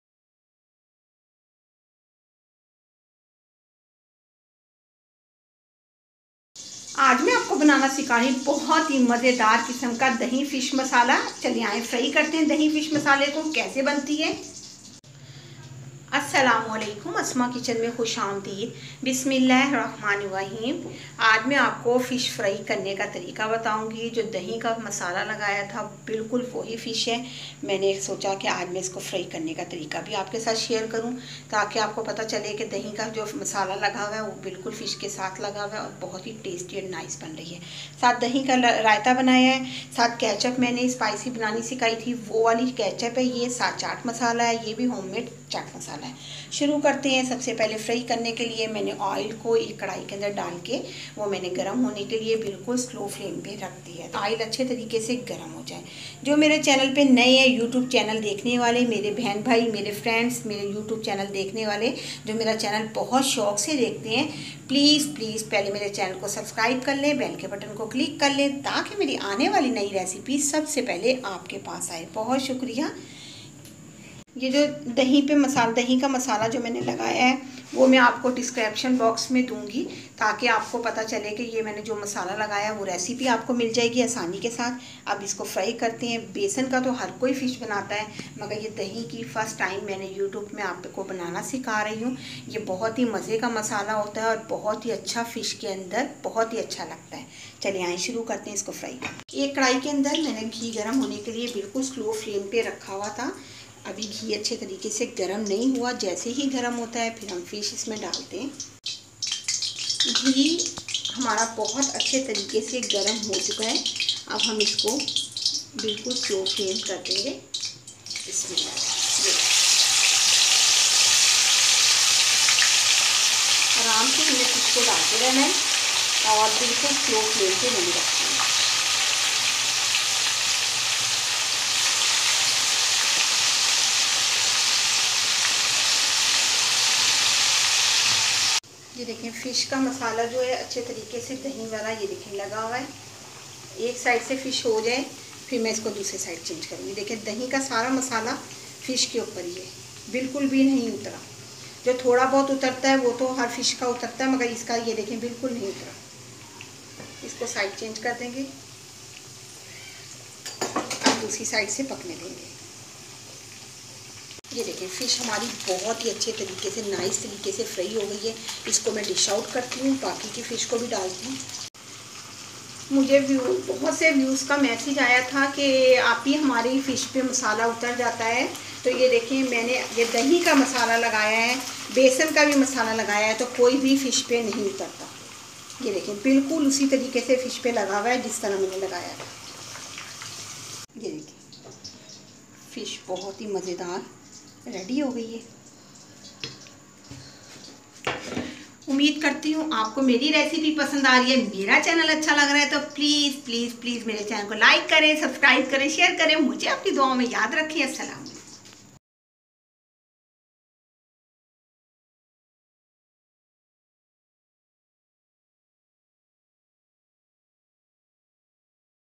Dahi wali Fish Fry karne ka Tarika دہی والی فش فرائی کرنے کا طریقہ
Dahi wali fish fry karne ka tarika
ingredients
2 kg fish
oil
ketchup
yougert
chaat masala

Dahi wali fish per masala lagane ka tarika